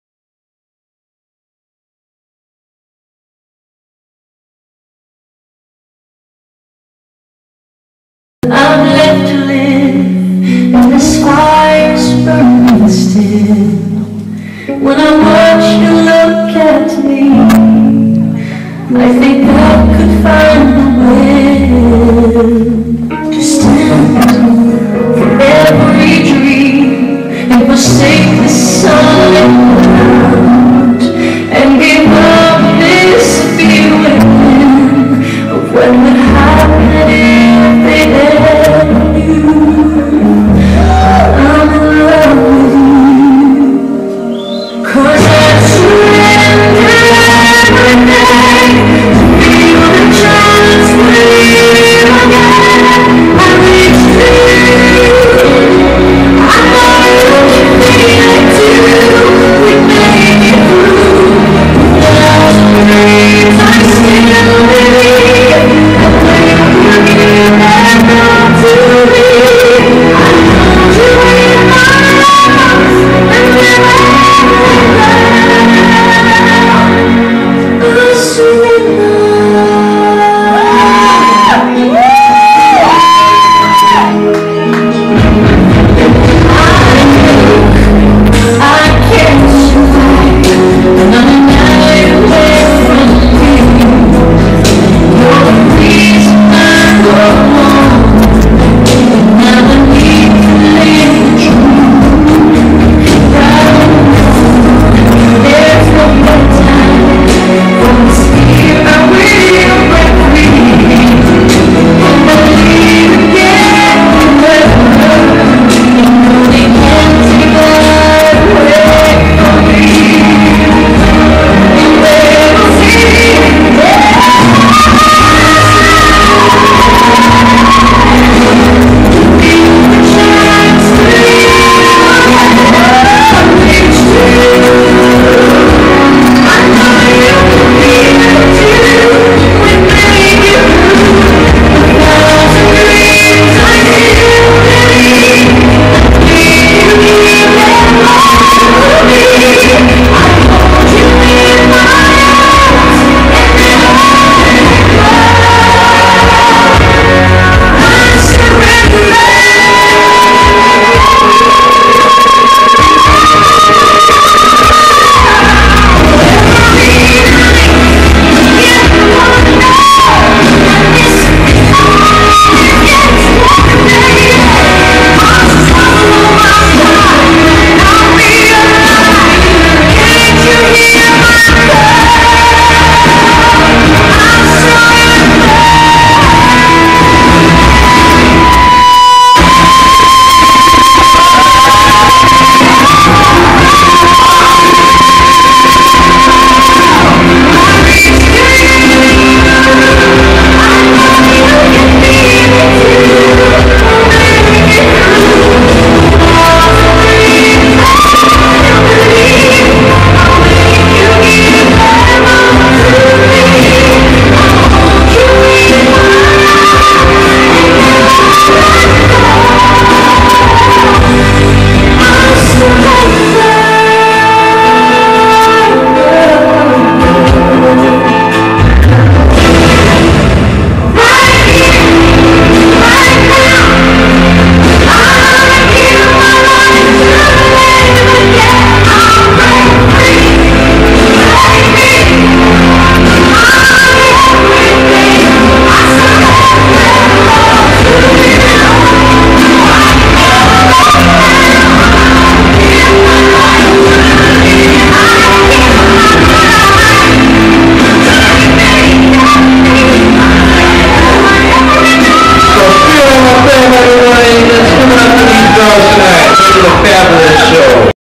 SAPHIRA lA'FEMME @ TRIANGLES CAFE
Saphira La'Femme performing at Triangles Cafe in Danbury , CT.